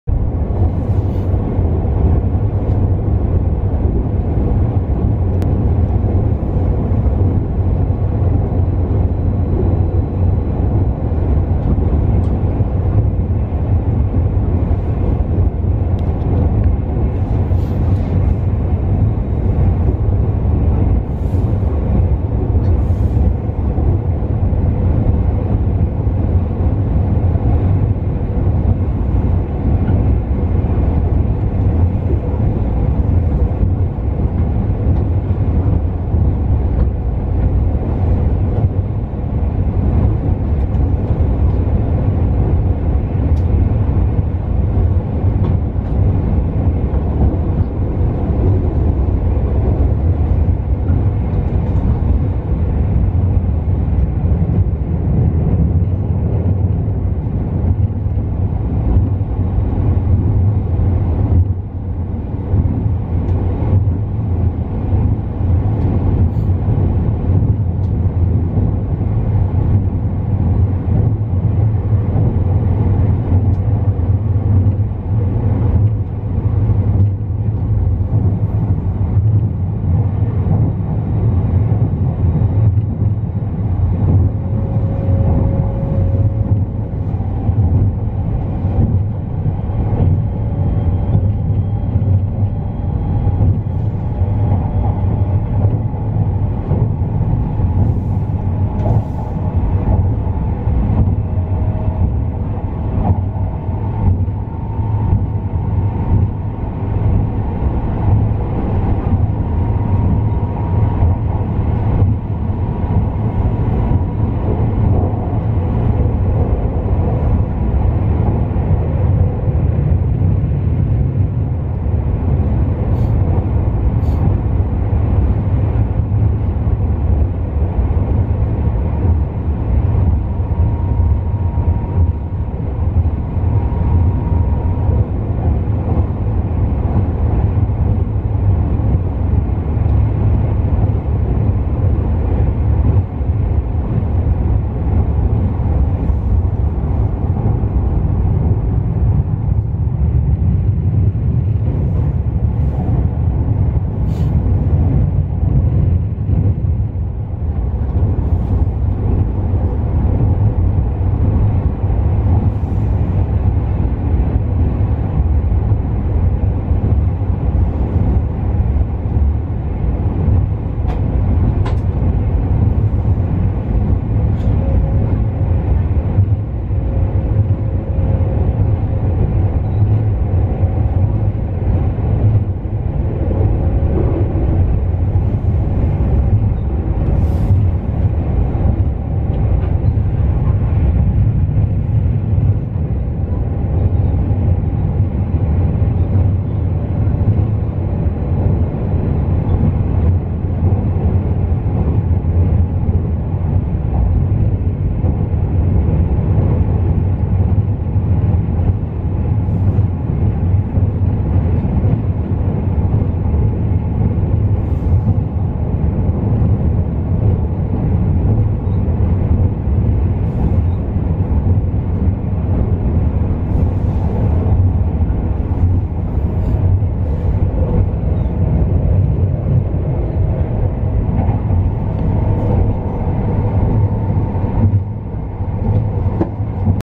寝台特急サンライズが瀬戸大橋を渡る様子。
だいぶ前に撮影しました。